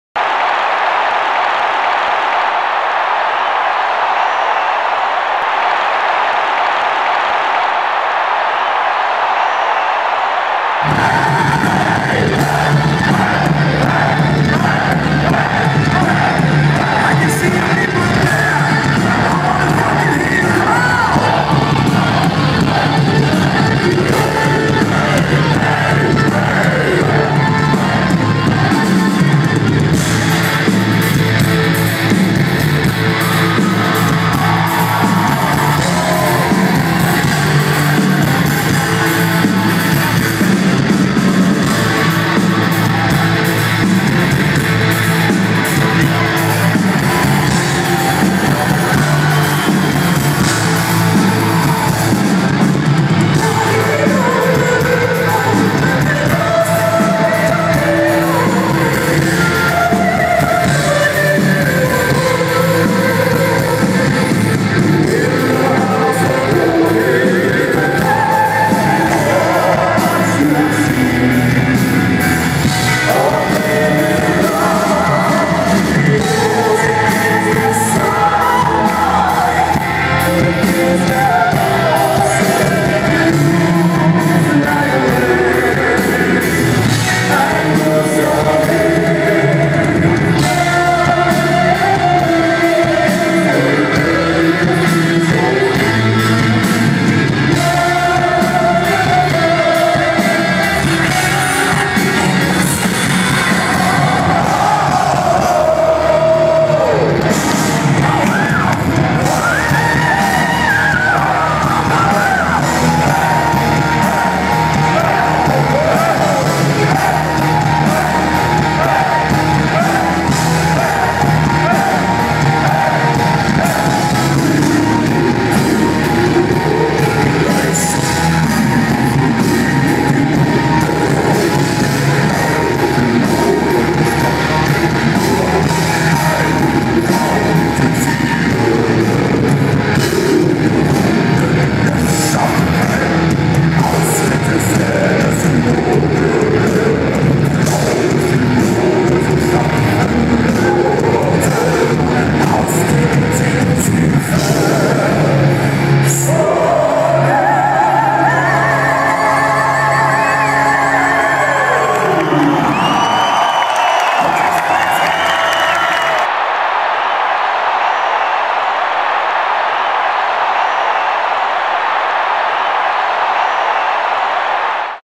Therion en Bogota Junio 2012 Orbita Rock
Presentacion del pasado 7 de Junio en el Royal Center de la ciudad de Bogota, encuentra la reseña y la galeria de fotos